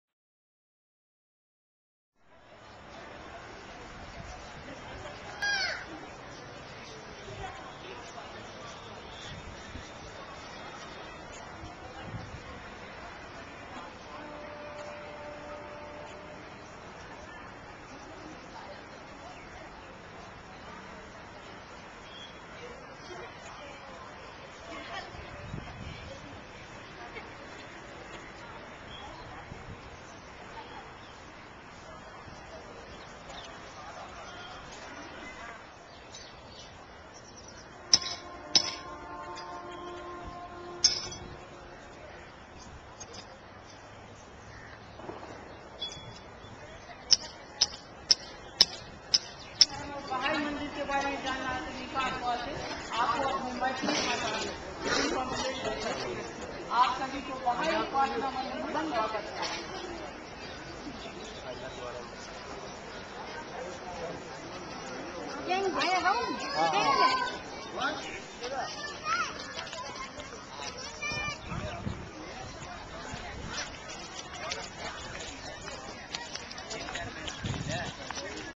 The Lotus Temple Baha'i House of Worship Delhi, India 2009 Oct
A visit to the Lotus Temple in Delhi, India.  The peaceful site was developed by Canadian/Iranian architect Fariborz Sahba.